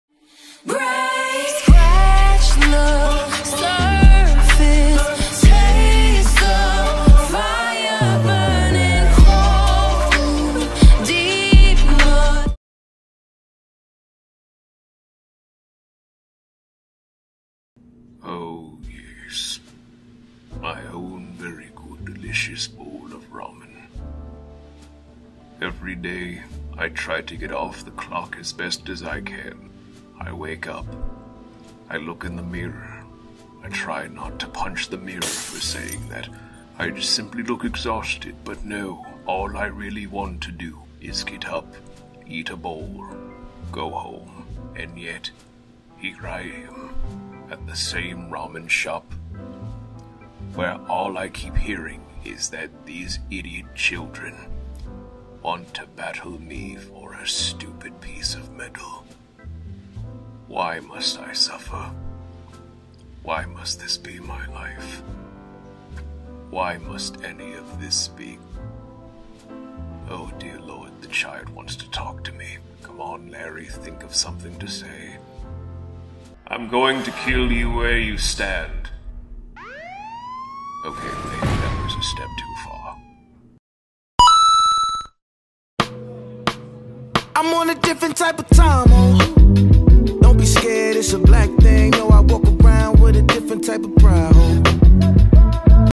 Voice Acting Attempt Round 1: Larry (Pokemon Scarlet & Violet) || Vincent
Round 1:
Larry - Normal Gym Leader from Pokemon Scarlet & Pokemon Violet

Voice Direction:
Tired & Fed-Up Salaryman looking for peace